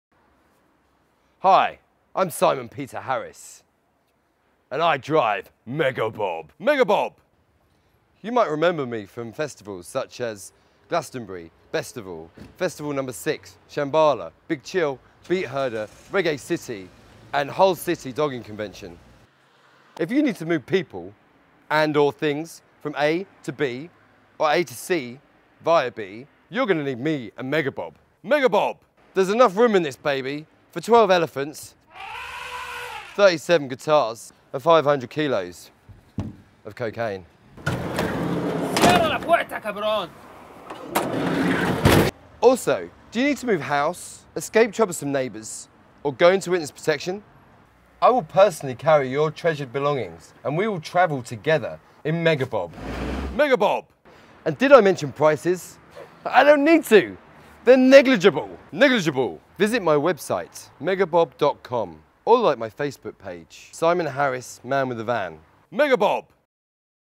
Simon Harris Man With A Van - Mega Bob
Advert for Simon Harris Man With A Van - Mega Bob - London-based minibus for hire.